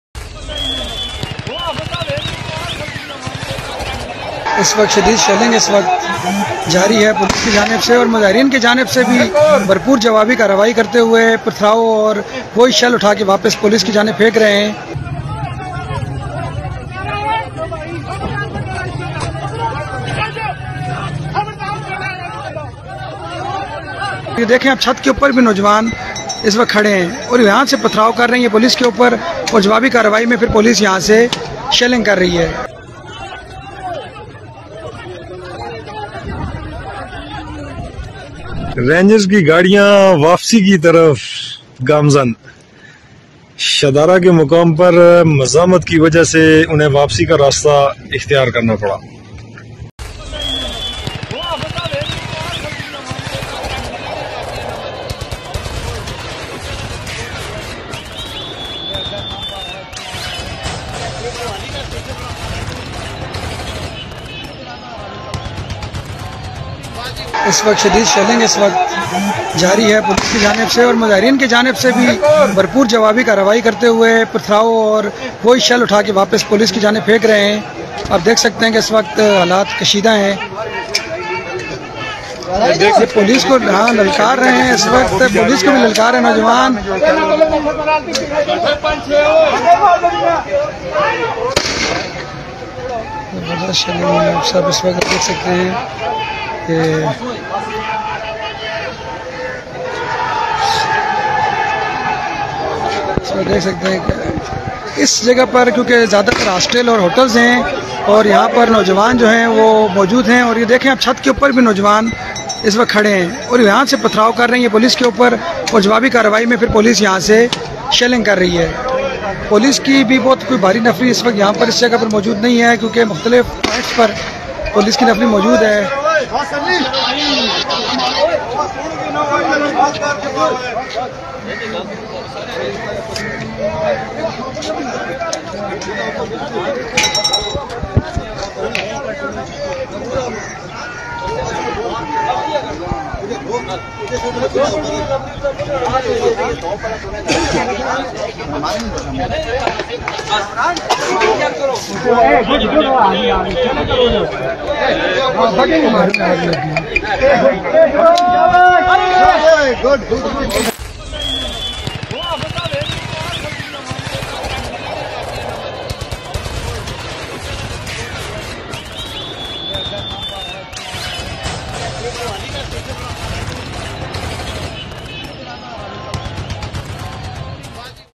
Posters Demand PoK Merger With India Amid Protests: Is Pakistan Losing Grip?
"In Pakistan-occupied Jammu and Kashmir, a burgeoning rights movement sees locals protesting perceived brutality by Pakistani authorities. Protests and clashes erupt in Muzaffarabad and Rawalakot, with calls for justice and fair governance resonating throughout the region. The demonstrations underscore mounting discontent and resistance, casting doubt on Pakistan's control over the disputed territory."